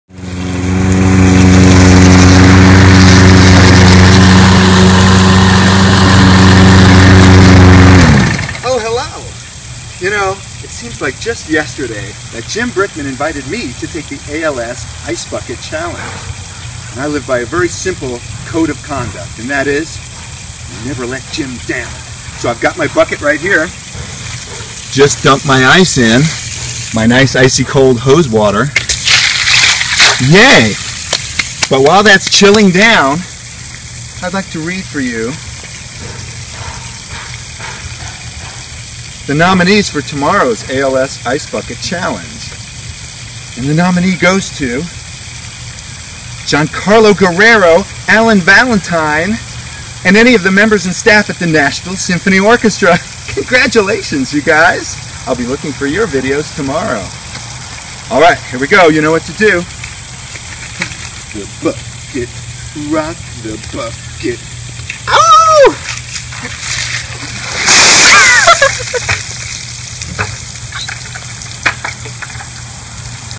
Tracy Silverman ALS Ice Bucket Challenge
Tracy Silverman takes the ALS Ice Bucket Challenge